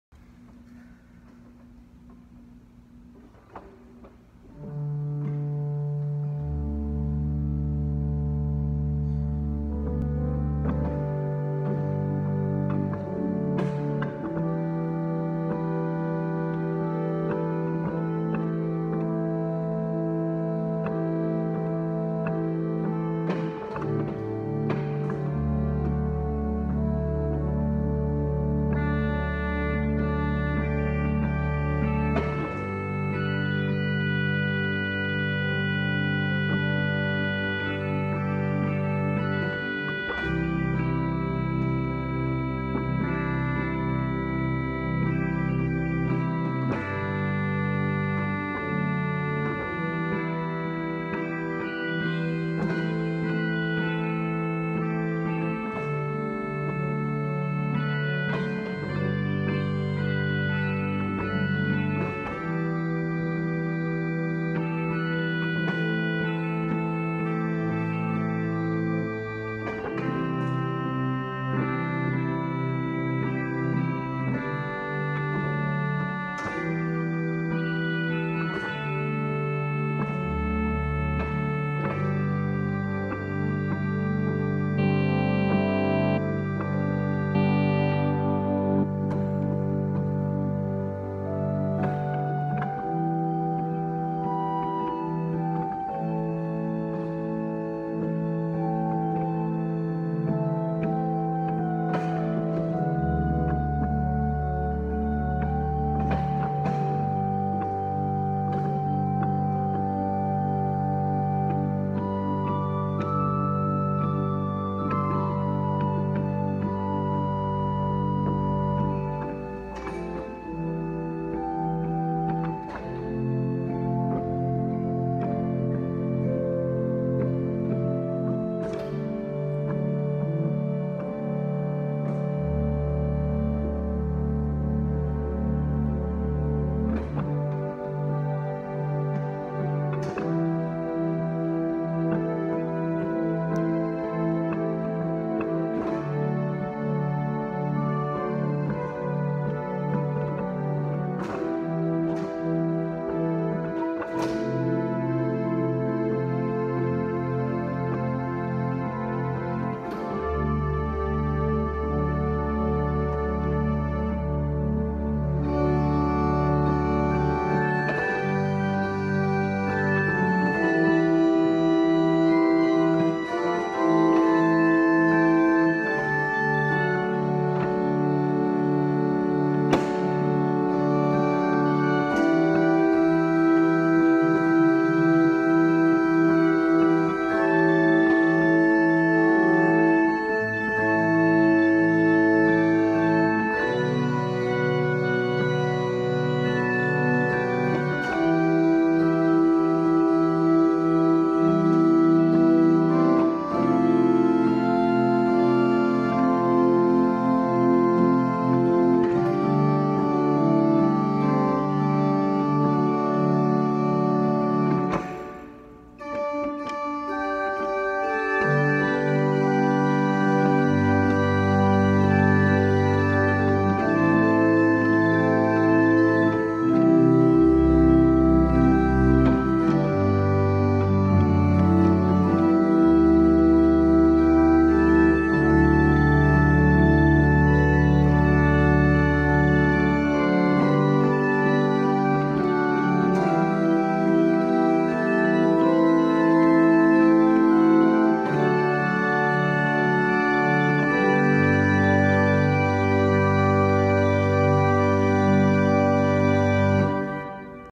Playing around with the organ
Trying to see what I can make this little, but big wonder do